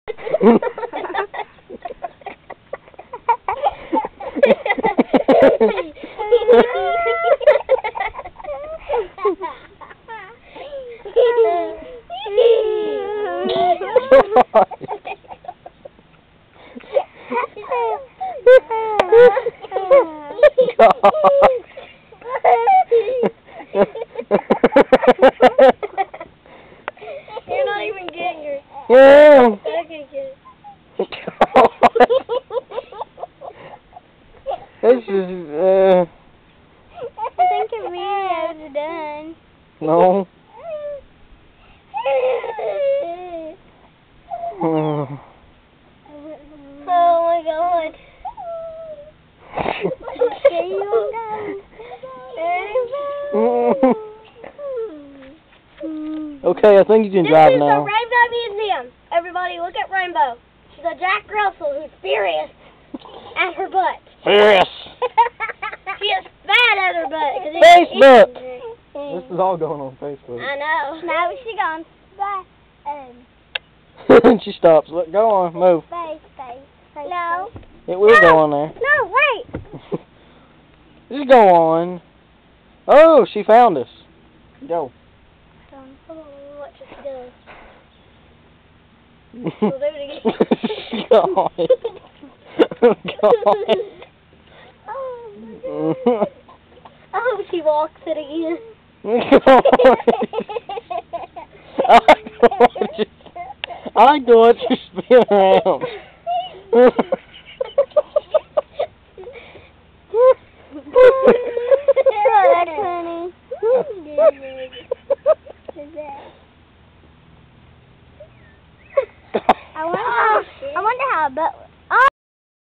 rainbow butt scratch
Rainbow (a dog of course) scratchin her butt.